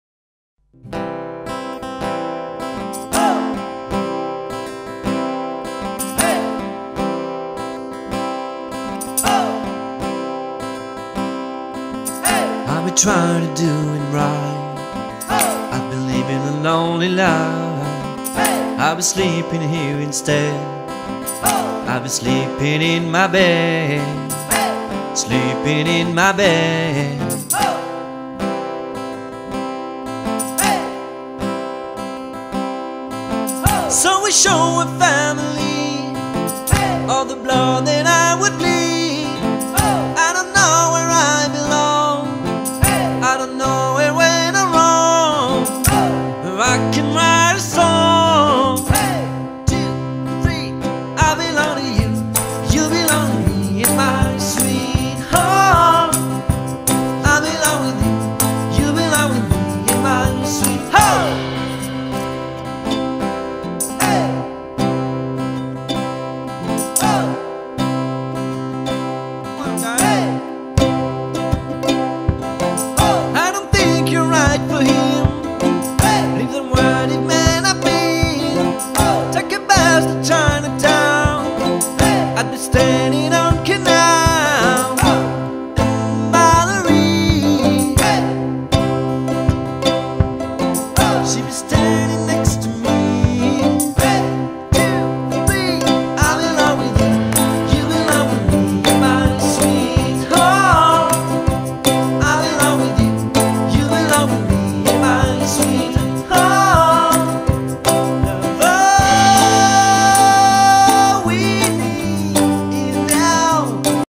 3H de régate à st pardoux 12-10-2013
35 voiliers au départ.